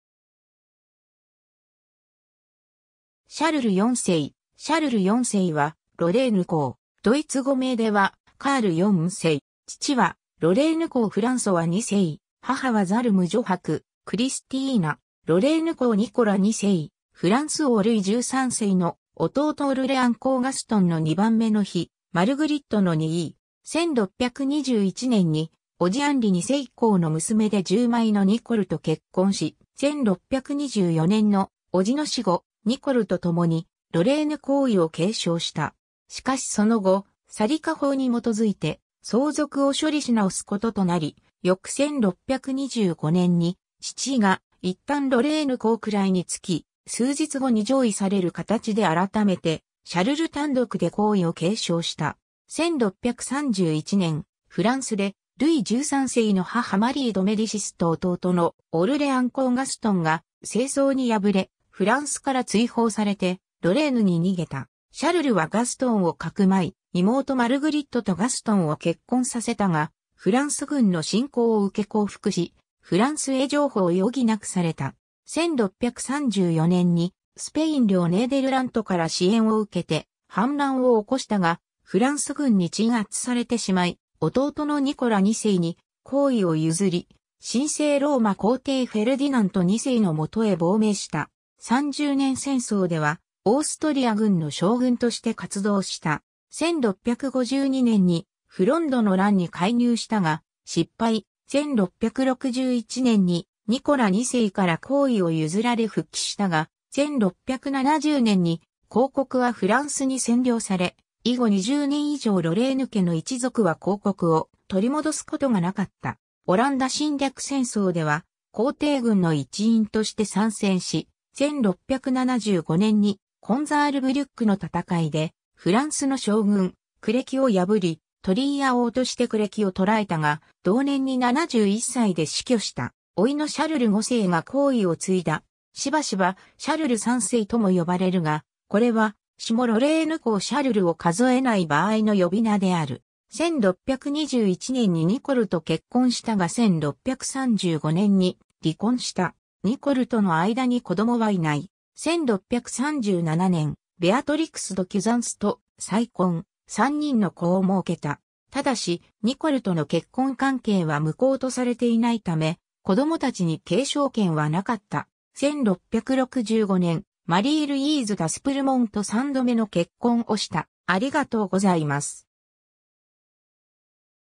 シャルル4世 (ロレーヌ公)
シャルル4世 シャルル4世（Charles IV, 1604年4月5日 - 1675年9月18日）は、ロレーヌ公（在位：1624年 - 1625年、1625年 - 1634年、1661年 - 1675年）。
ドイツ語名ではカール4世（Karl IV）。
父はロレーヌ公フランソワ2世、母はザルム女伯クリスティーナ。
ロレーヌ公ニコラ2世、フランス王ルイ13世の弟オルレアン公ガストンの2番目の妃マルグリットの兄。
 1621年に伯父アンリ2世公の娘で従妹のニコルと結婚し、1624年の伯父の死後、ニコルと共にロレーヌ公位を継承した。
しかしその後、サリカ法に基づいて相続を処理しなおすこととなり、翌1625年に父が一旦ロレーヌ公位に即き、数日後に譲位される形で改めてシャルル単独で公位を継承した。
 1631年、フランスでルイ13世の母マリー・ド・メディシスと弟のオルレアン公ガストンが政争に敗れ、フランスから追放されてロレーヌに逃げた。
シャルルはガストンを匿い、妹マルグリットとガストンを結婚させたが、フランス軍の侵攻を受け降伏し、フランスへ譲歩を余儀なくされた。
1634年にスペイン領ネーデルラントから支援を受けて反乱を起こしたが、フランス軍に鎮圧されてしまい、弟のニコラ2世に公位を譲り、神聖ローマ皇帝フェルディナント2世の下へ亡命した。
三十年戦争ではオーストリア軍の将軍として活動した。
 1652年にフロンドの乱に介入したが失敗、1661年にニコラ2世から公位を譲られ復帰したが、1670年に公国はフランスに占領され、以後20年以上ロレーヌ家の一族は公国を取り戻すことがなかった。
オランダ侵略戦争では皇帝軍の一員として参戦し、1675年にコンザールブリュックの戦いでフランスの将軍クレキを破り、トリーアを落としてクレキを捕らえたが、同年に71歳で死去した。
甥のシャルル5世が公位を継いだ。
 しばしば「シャルル3世」とも呼ばれるが、これは下ロレーヌ公シャルルを数えない場合の呼び名である。
 1621年にニコルと結婚したが1635年に離婚した（教皇庁から婚姻の無効は認められていない）。
ニコルとの間に子供はいない。
 1637年、ベアトリクス・ド・キュザンス（1614年 - 1663年）と再婚、3人の子を儲けた。
ただし、ニコルとの結婚関係は無効とされていないため、子供達に継承権は無かった。
 1665年、マリー・ルイーズ・ダスプルモン（1652年 - 1692年）と3度目の結婚をした。